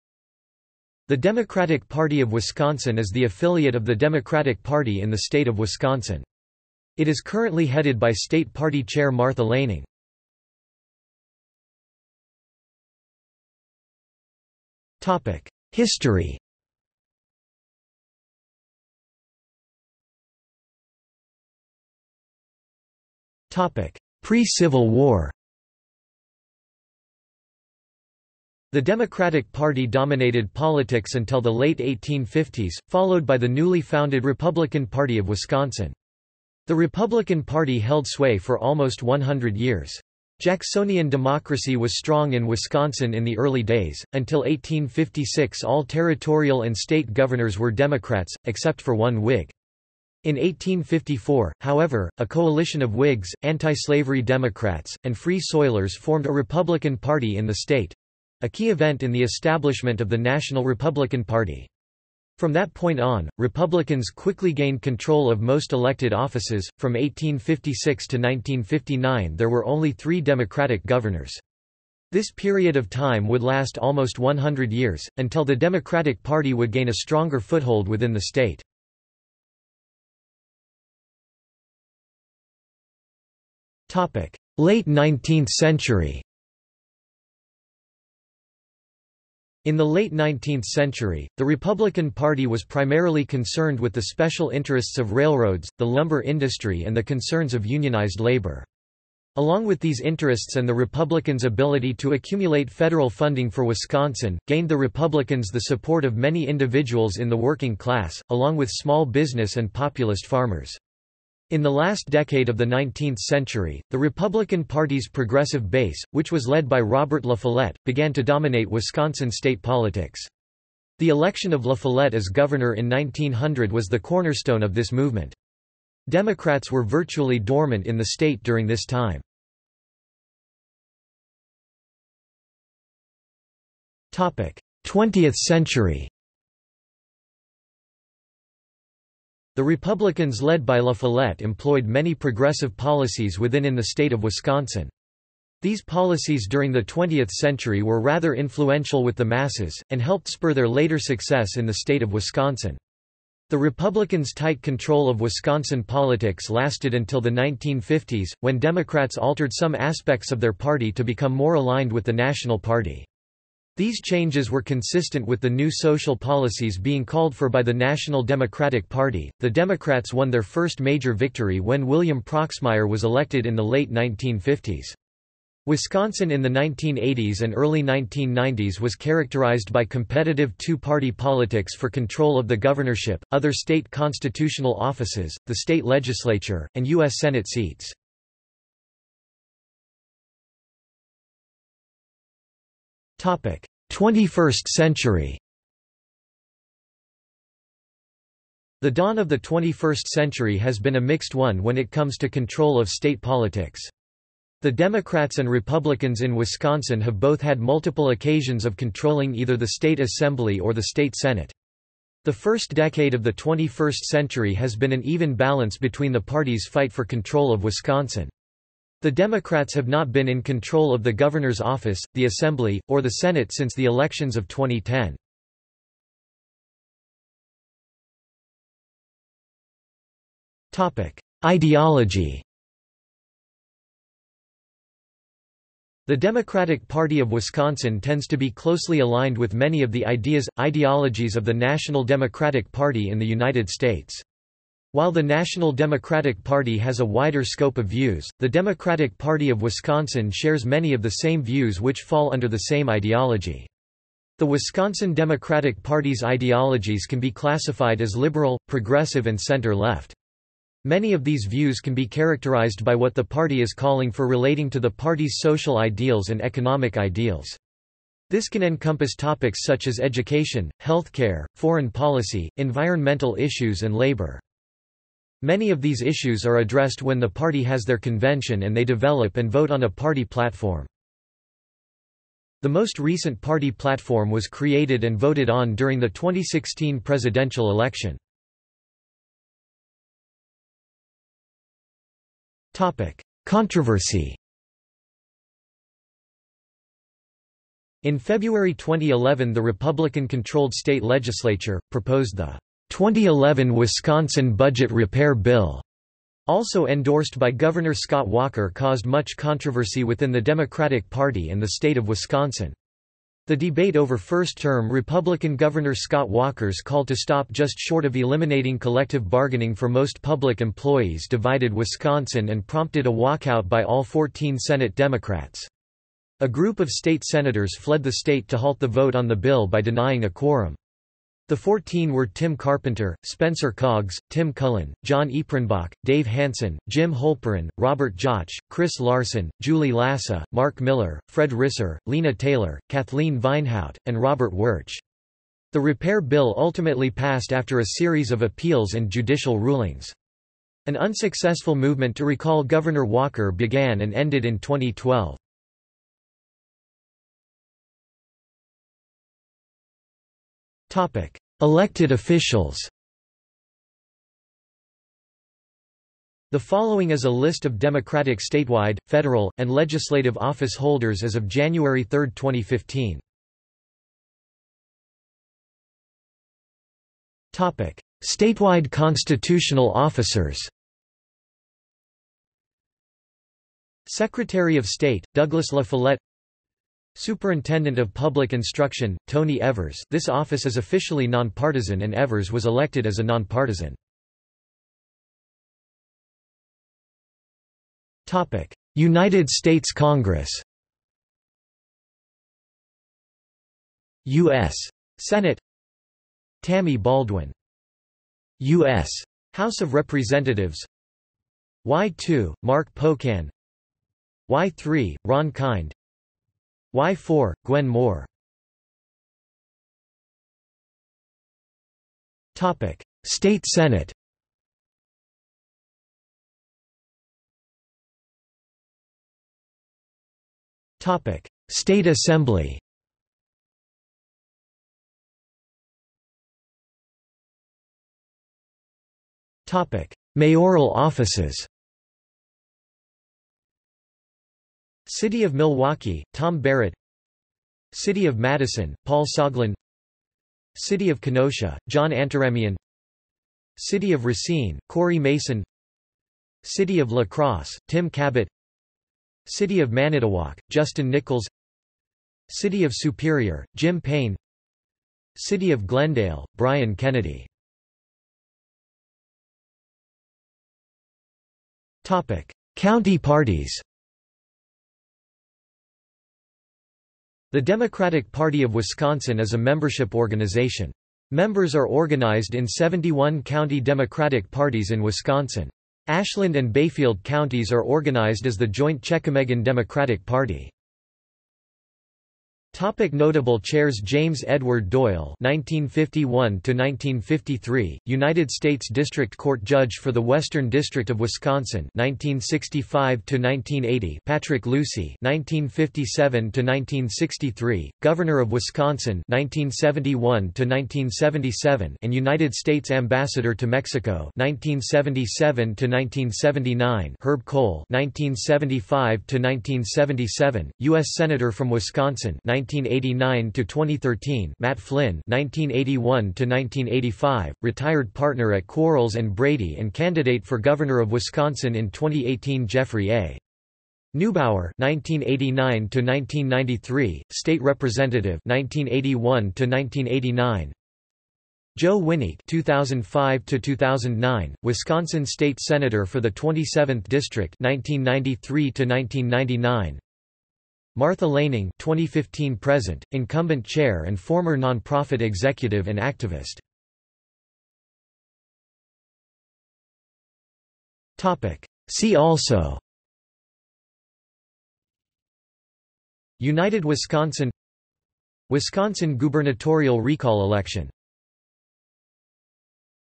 Democratic Party of Wisconsin | Wikipedia audio article
This is an audio version of the Wikipedia Article:
Democratic Party of Wisconsin

00:00:11 1 History
00:00:20 1.1 Pre-civil war
00:01:20 1.2 Late 19th century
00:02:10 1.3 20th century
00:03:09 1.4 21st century
00:03:47 2 Ideology
00:04:49 3 Controversy
00:05:57 4 Elected officials
00:06:14 4.1 Statewide constitutional officers
00:06:34 4.2 United States Congress
00:06:44 4.2.1 U.S. Senate
00:06:48 4.2.2 U.S. House of Representatives
00:06:59 4.3 State Senate
00:07:07 4.4 State Assembly
00:07:16 4.5 Mayoral offices
00:07:48 5 County parties
00:08:12 6 Notable chairs
00:09:17 7 See also

- Socrates